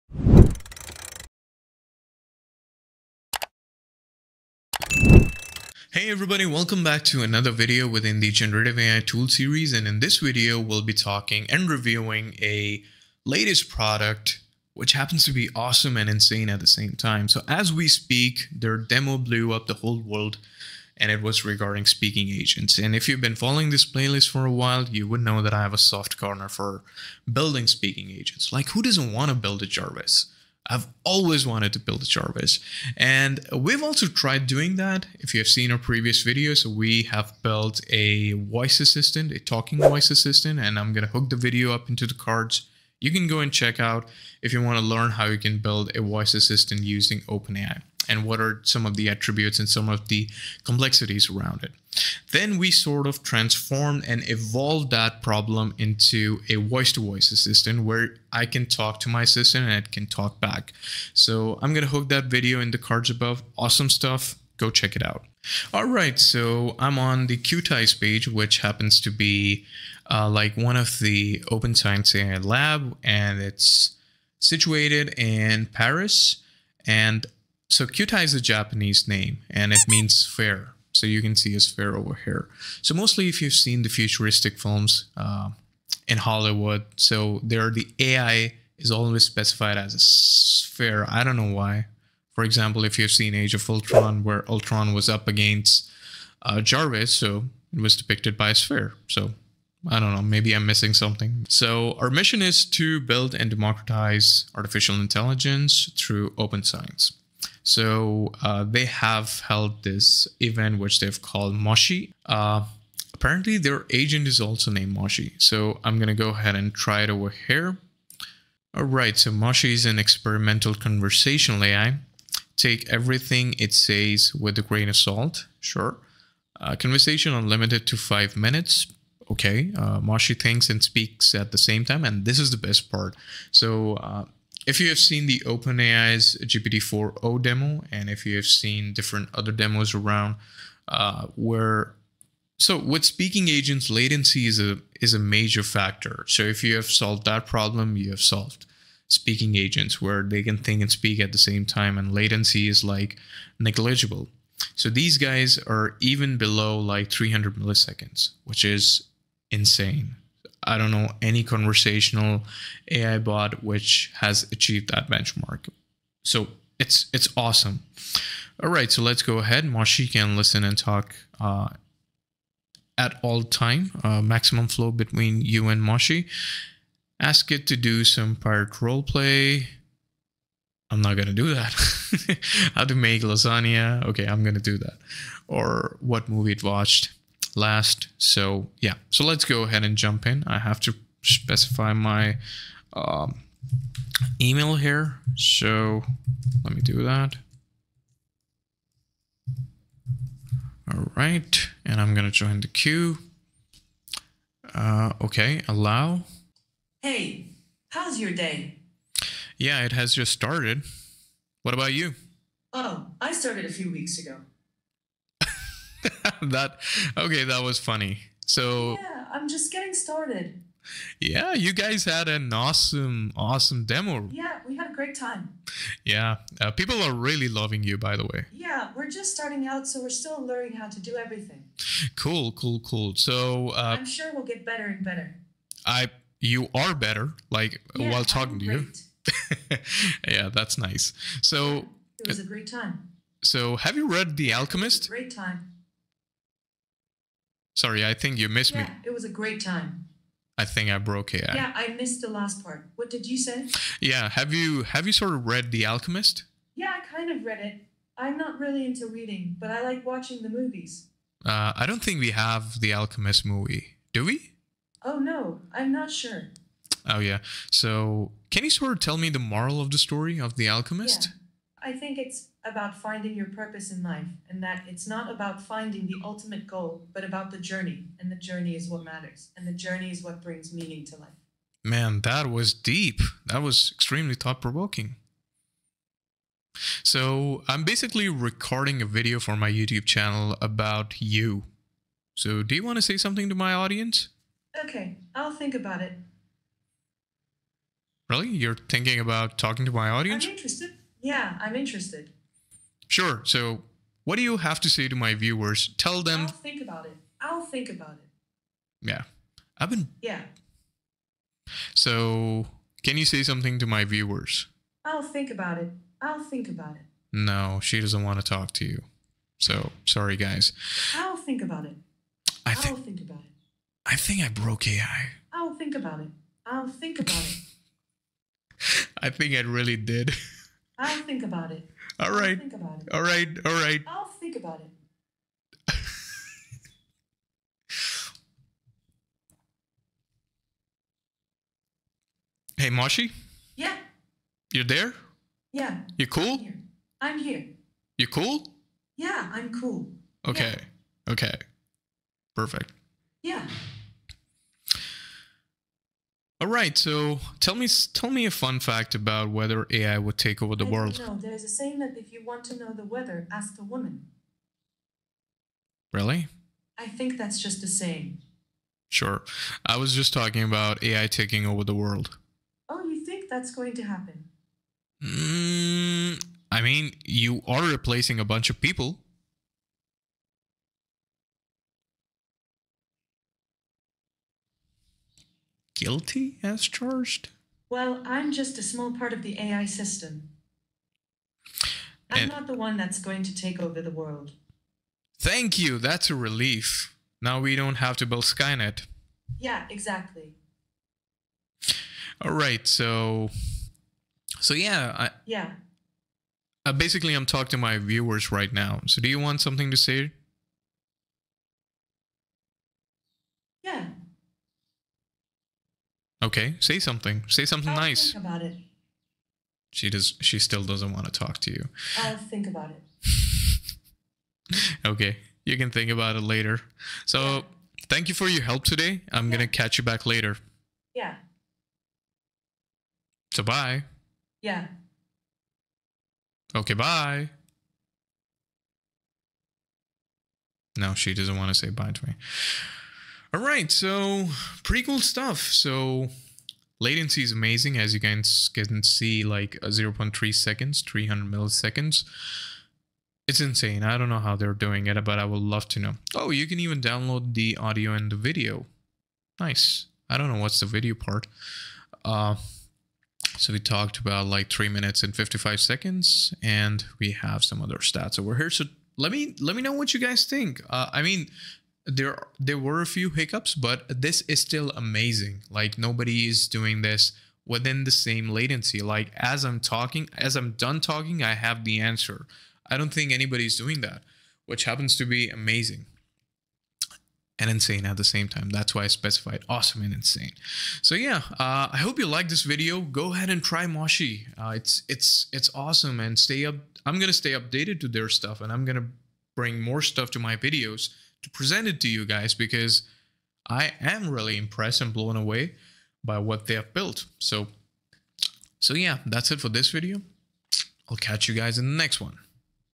KYUTAI MOSHI - The Fastest AI Speaking Agent | Generative AI Tools | Bits & Bytes # 8
Discover the groundbreaking AI innovation from kyutai Labs with “Moshi,” their advanced talking agent. In this video, we delve into Moshi’s capabilities and how it leverages cutting-edge artificial intelligence to enhance user interaction. Kyutai, Europe’s pioneering AI open-science lab, focuses on developing large multimodal models incorporating text, sound, and images to push the boundaries of AI research. With substantial backing from industry leaders like the iliad Group, CMA CGM Group, and Schmidt Futures, Kyutai is set to revolutionize the AI landscape. Join us as we explore the features and potential of Moshi in transforming communication and accessibility.

and stay updated with the latest in AI research and development.